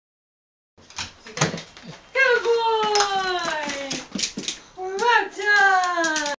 cody let me in
Cody the Dalmation letting himself in through the back door